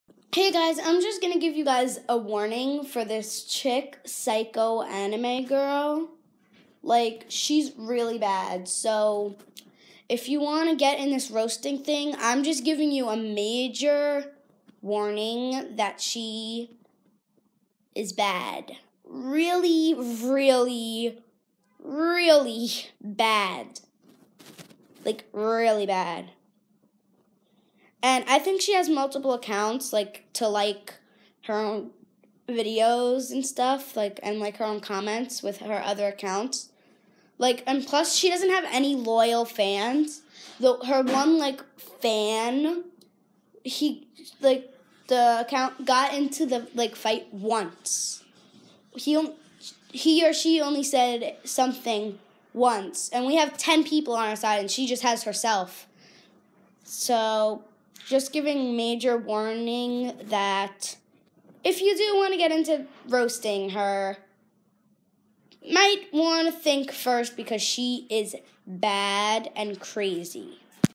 WATCH OUT FOR PHSCHO ANIME GIRL (read disc)
MAJOR WARNING ⚠️ PSYCHO ANIME GIRL CURSES EVERYONE OUT AND SHE IS CRAZYYYYY